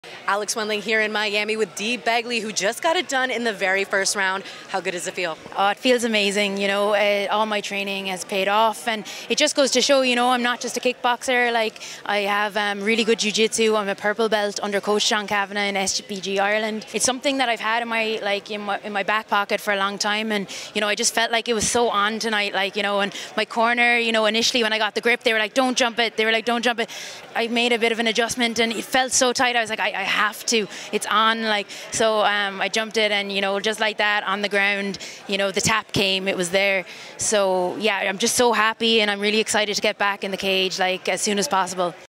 Dee Begley’s arm-in guillotine vs Hannah Guy’s single leg attempt Combate Global Mujeres
Dee Begley went against her coach's advice and JUMPED GUARD when she secured the arm in guillotine grip! This was a great comeback fight for her after being knocked out cold by No. 1 ranked flyweight Maritza Sanchez. SBG IRELAND FIGHTERS are coming in 2023 hot!

Conor McGregor’s teammate & Combate Global flyweight Dee Begley SNATCHED A NECK in the first round of her comeback fight🔥🇮🇪